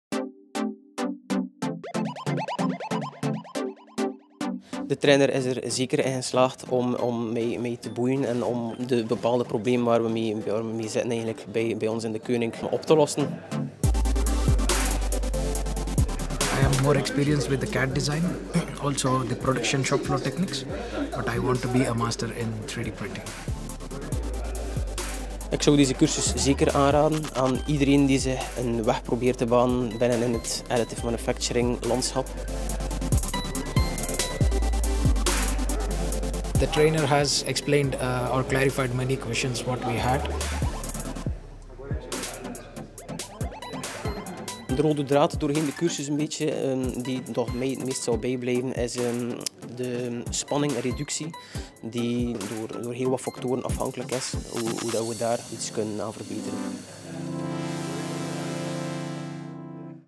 Masterclass in Metal 3D Printing
Getting started with Metal 3D Printing? Regardless of the industry, you operate in, success in Metal 3D Printing depends on your mastery of the printing process. If you’re looking to understand the various factors which come into play in Metal AM, and how to manage these factors, we’ve got you covered.

The Masterclass in Metal 3D Printing is a two-day training at Materialise HQ designed to shorten the learning curve for Metal AM and help you avoid build failures.